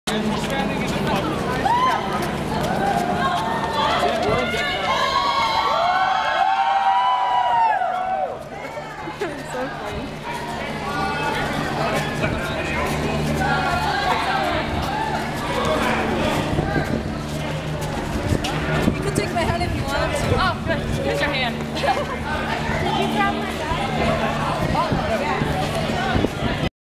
Anime Expo 2012 - Nintendo Gathering Train
Ok, so here's another vid where I'm cosplaying as Pit. XD
This was on Day 3 in the afternoon after the Nintendo Gathering picture shoots were done. All the Nintendo cosplayers got into a line and made a train back into the Convention Center.